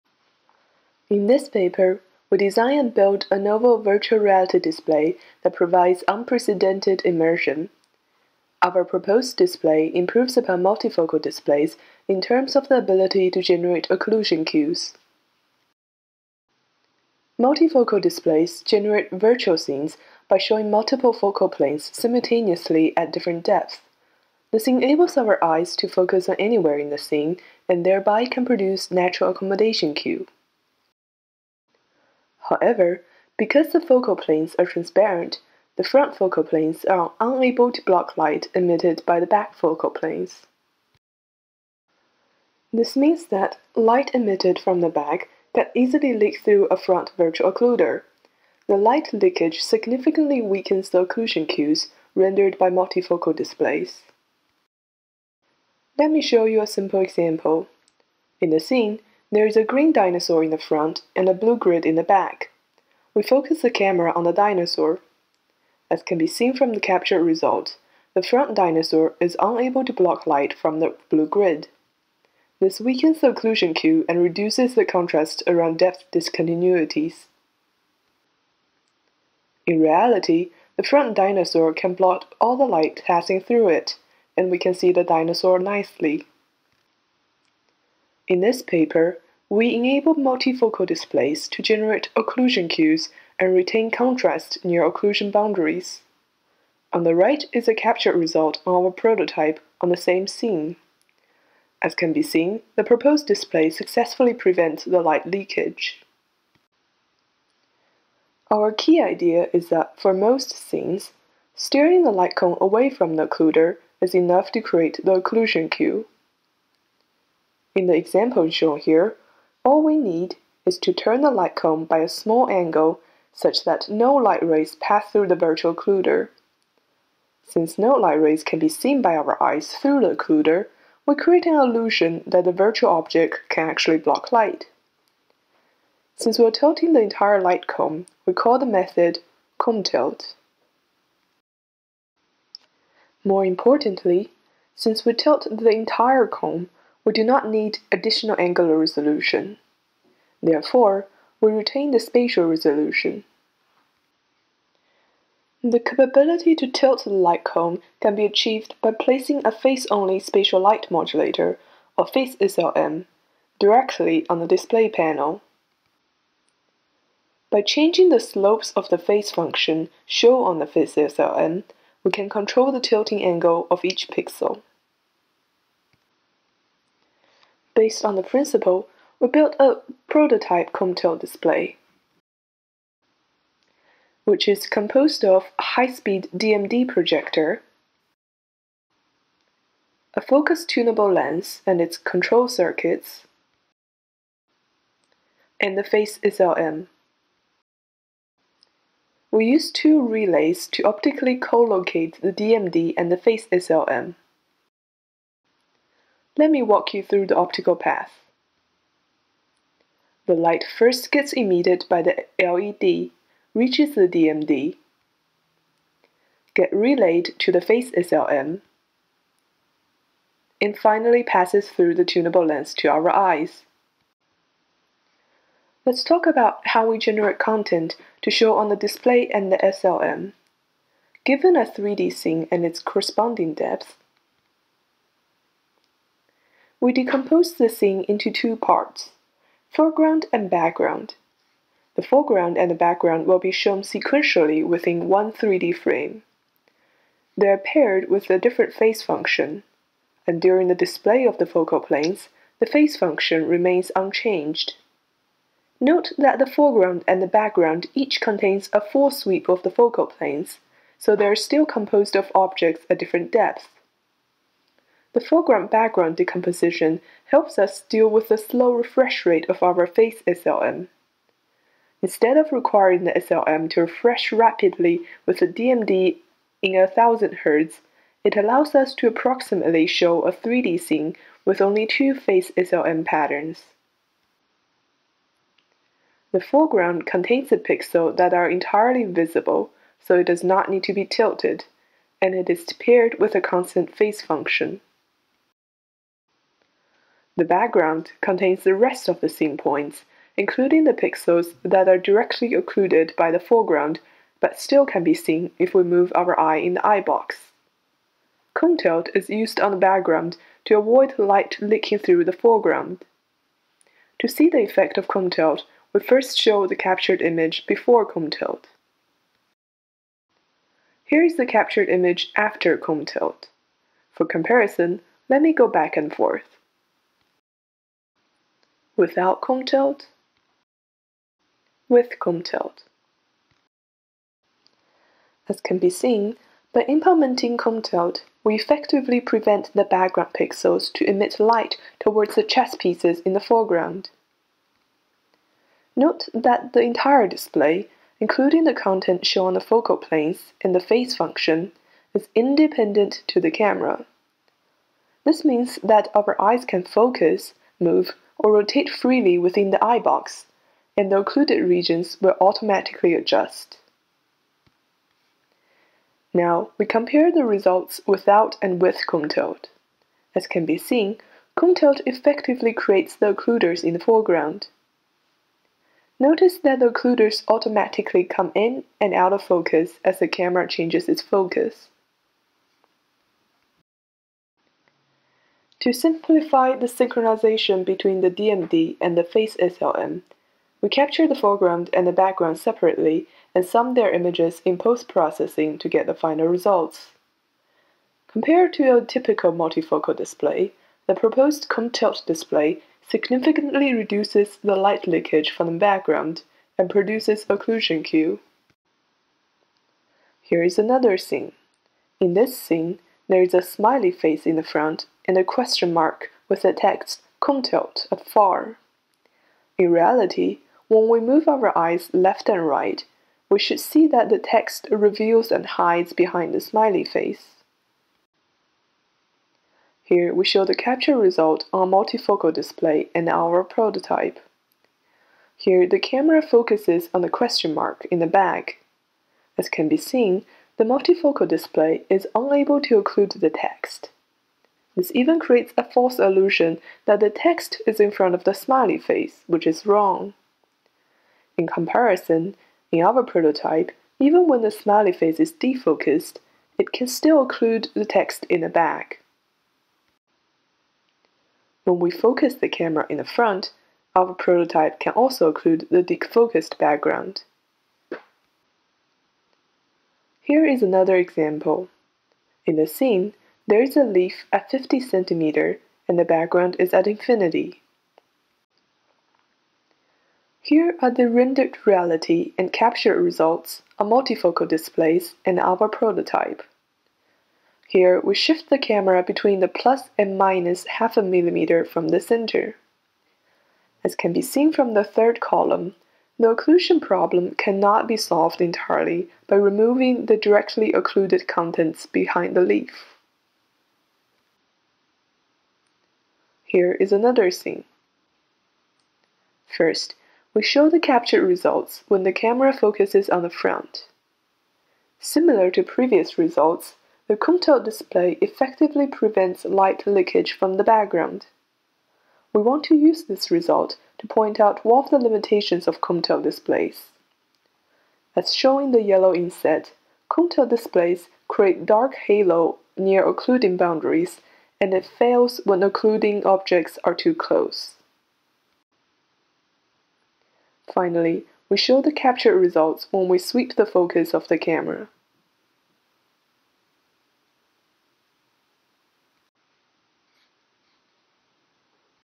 ConeTilt VR Display -- Towards Occlusion-aware Multifocal Displays [SIGGRAPH 2020]
Jen-Hao Rick Chang, Anat Levin, B. V. K. Vijaya Kumar, Aswin C. Sankaranarayanan
Siggraph 2020